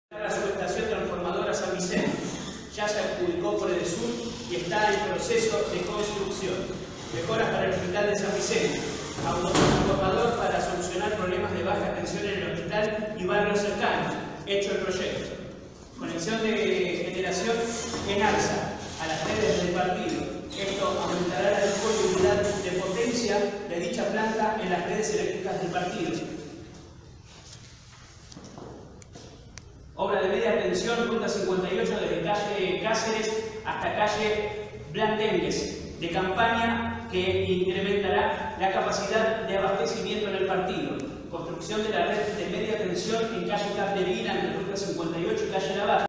Funcionarios de EDESUR daban una conferencia de prensa, se cortó la luz y los “atendieron”
Las autoridades de Edesur visitaron San Vicente. Junto al intendente Mauricio Gómez (Cambiemos) dieron una conferencia de prensa para hablar sobre los avances de obras y explicar el problema de energía que castiga y pone de malhumor a los vecinos. En medio de la exposición, en el salón principal del Municipio, el diablo metió la cola: se cortó la luz y todo quedó oscuras. Molesto, Gómez se los cargó uno por uno y acusó a la firma de no escuchar al vecino. Envalentonado, el presidente del Concejo Deliberante, lo segundeó en la embestida: “Yo los escucho y parece que todos los malos somos nosotros y  ustedes los buenos, los bonitos”, dijo Roberto Armirotti.